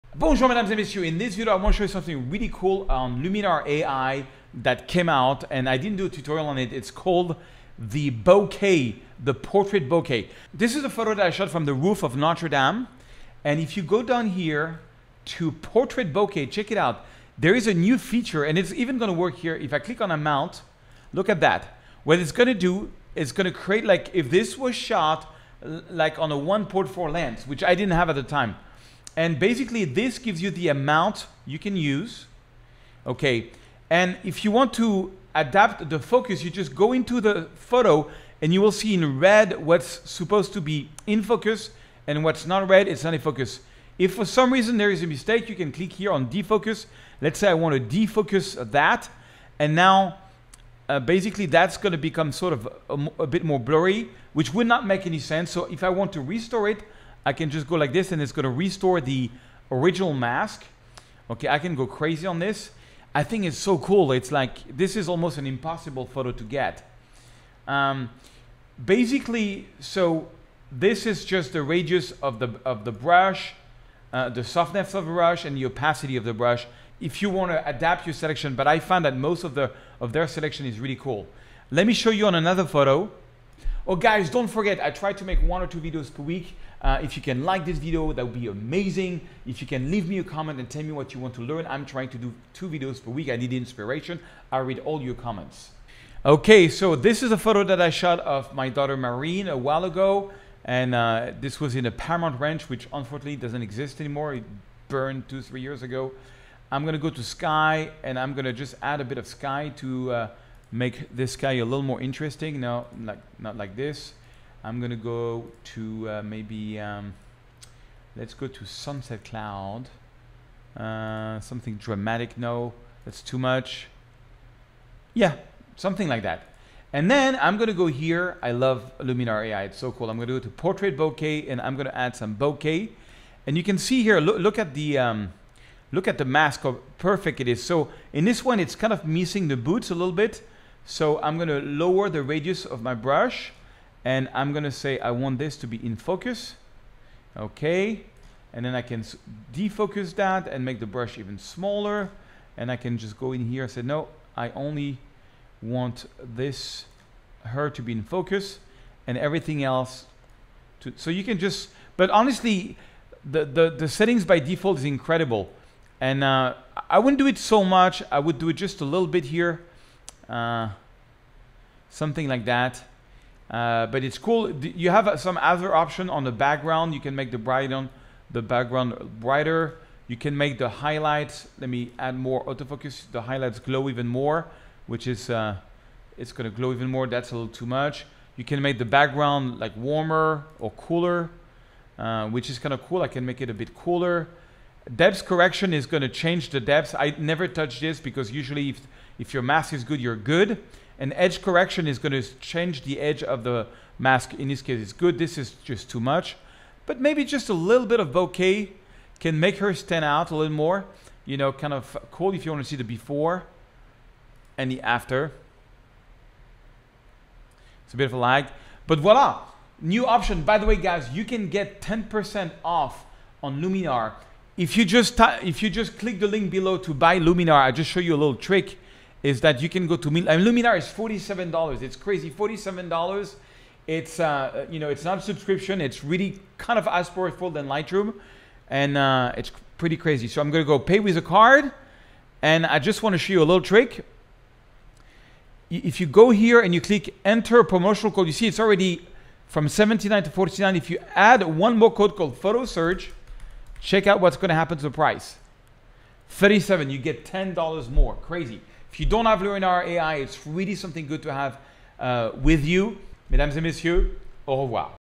Fake expensive lens with the NEW BOKEH filter in Luminar AI
00:10 How to add bokeh with Luminar - Notre Dame
00:33 Adapt the focus
01:37 Adding bokeh on a portrait in Luminar
01:45 Adding sky in Luminar
02:55 Other background options
03:41 Promo code Luminar